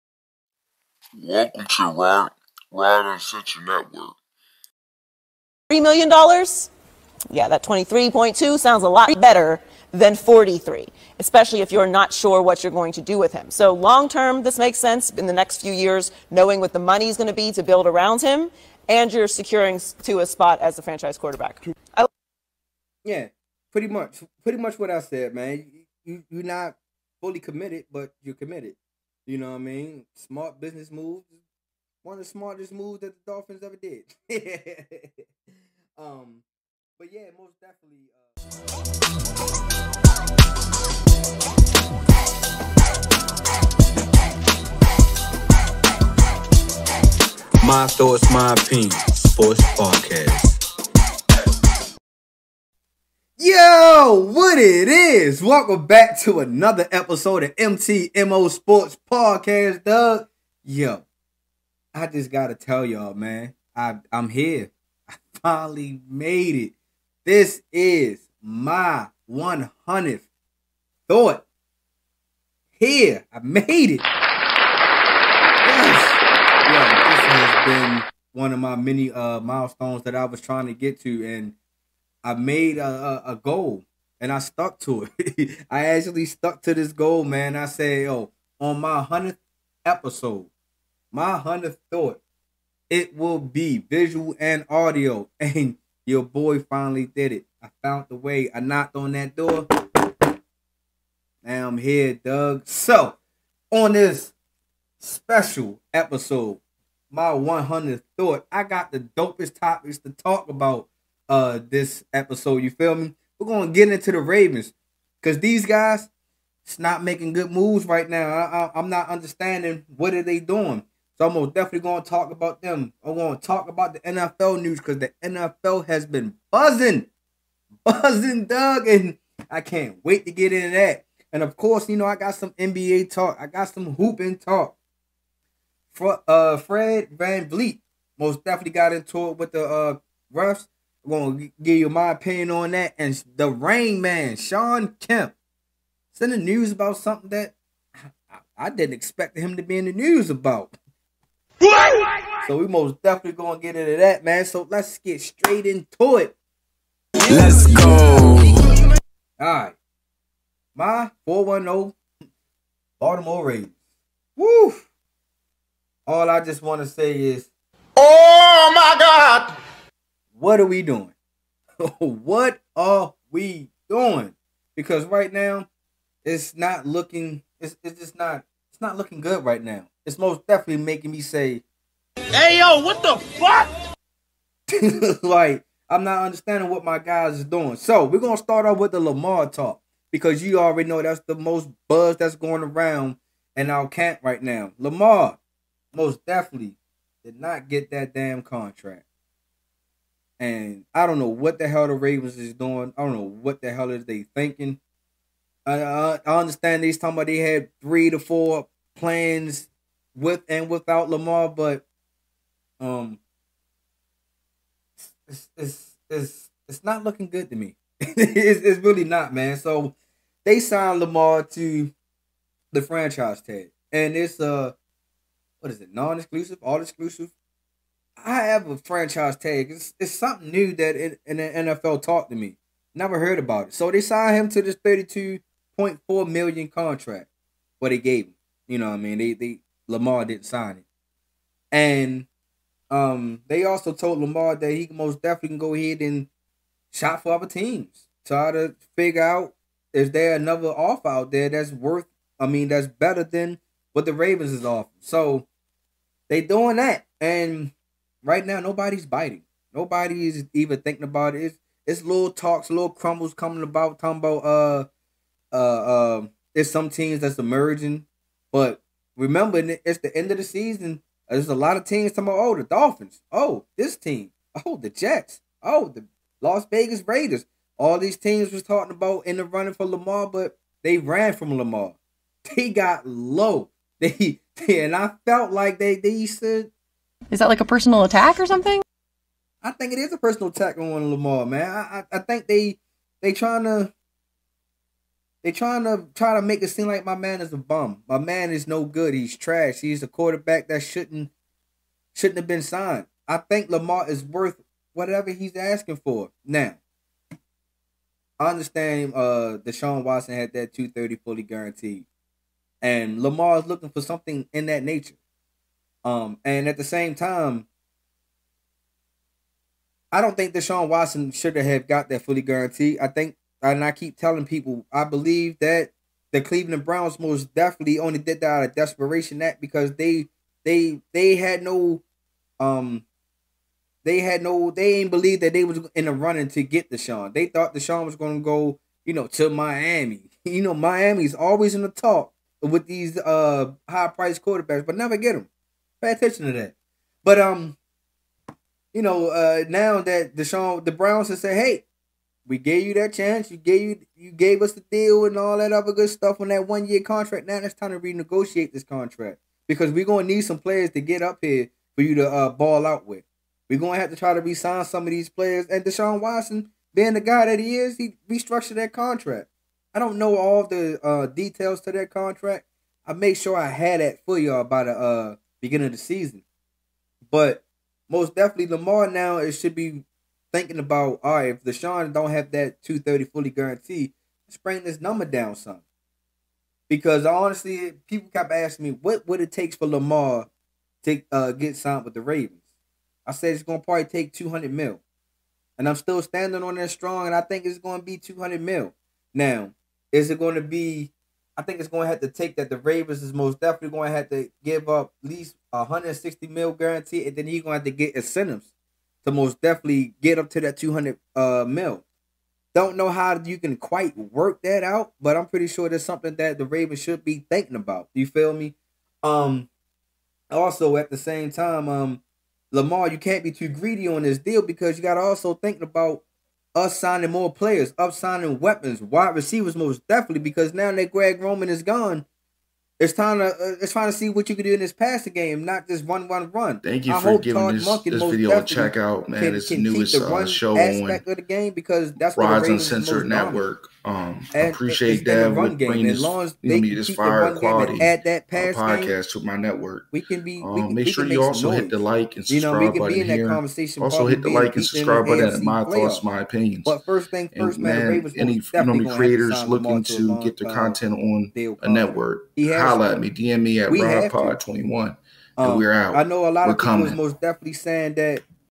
My thoughts My Opinion Sports Podcast : Lamar Jackson needs to be paid!!
Welcome to the team Devin ( host of MY Thoughts MY Opinion Sports Podcast.)